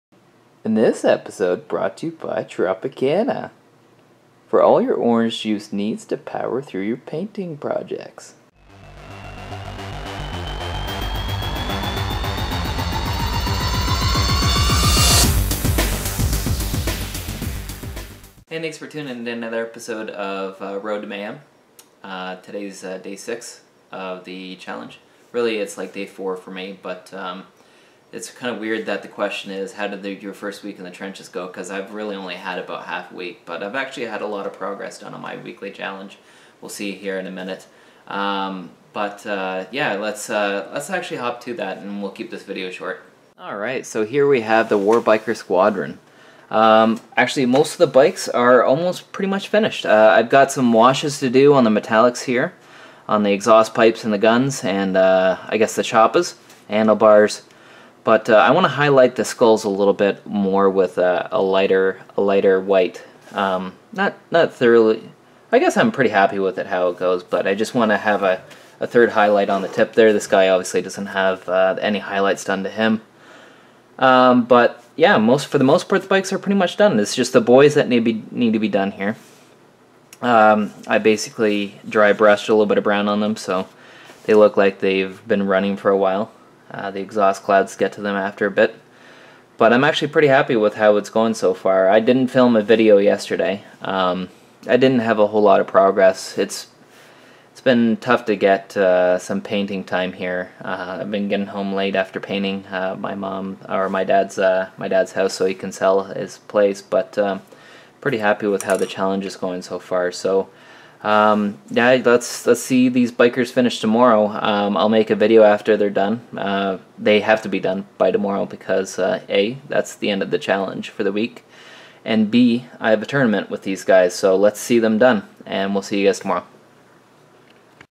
Road to mayhem Vlog - Ep. 4
A series following a painting challenge for the month of May.